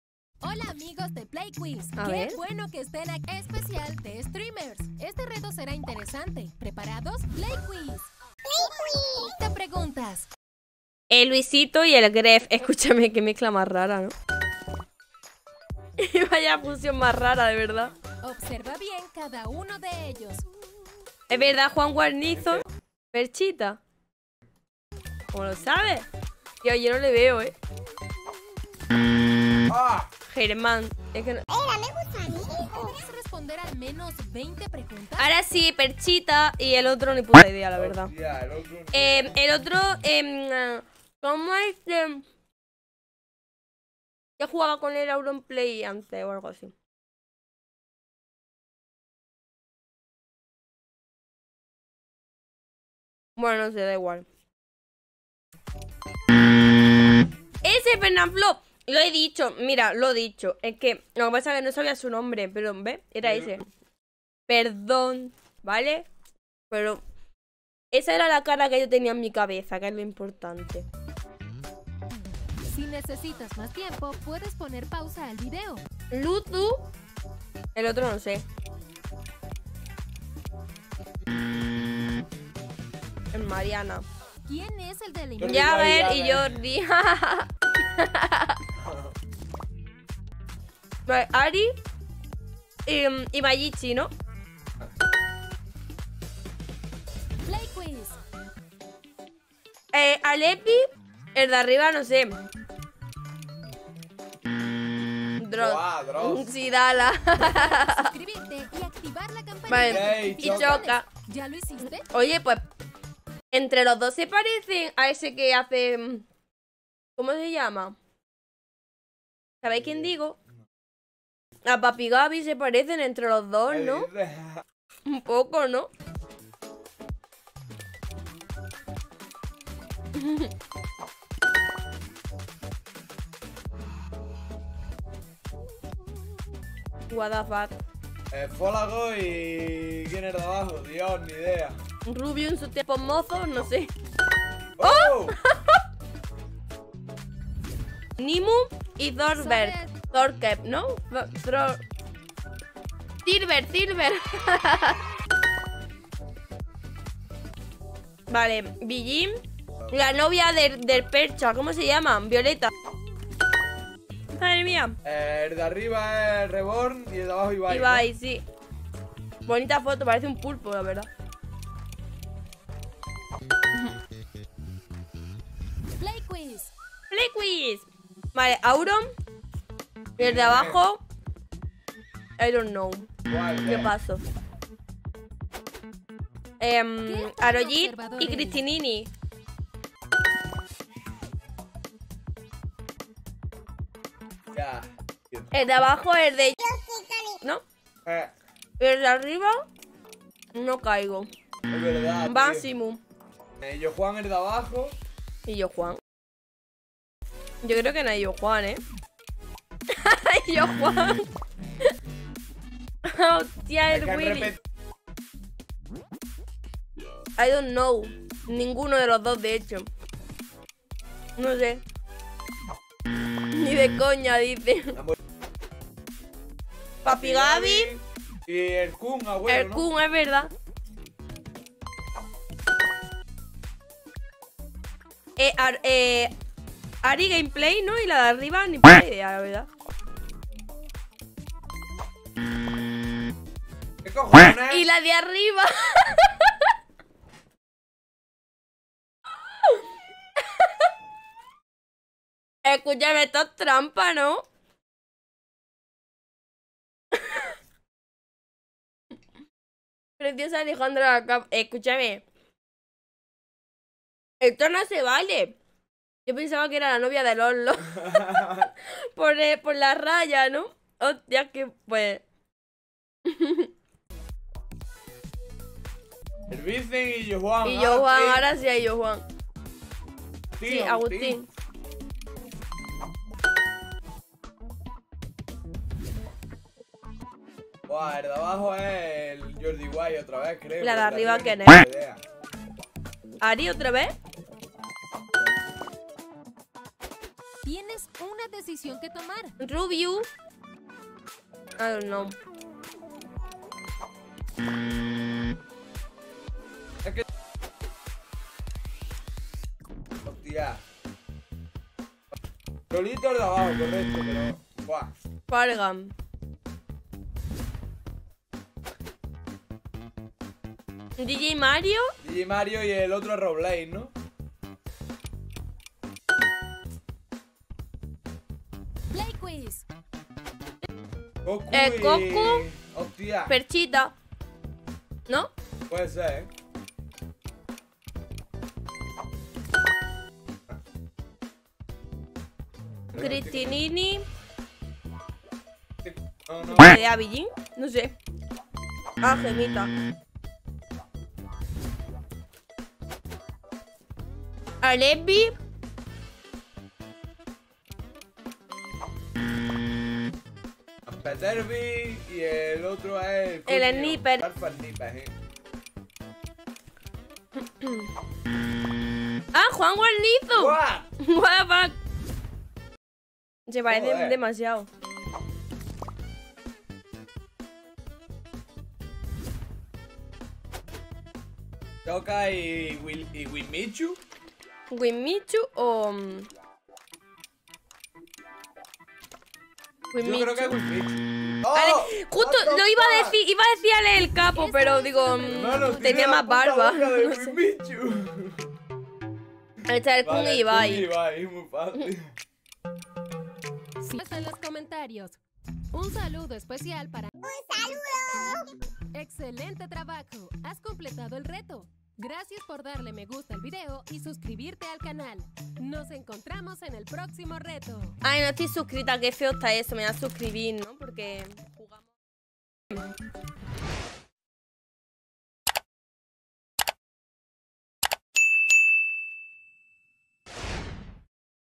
ADIVINA LOS 2 STREAMERS POR 👀 y 👄 | Reaccionando a PlayQuiz
Reacción a Play Quiz en directo, Adivina los dos streamers por ojos y boca
Al final no se dio tan bien como pensaba... 15/30, un suficiente 🥲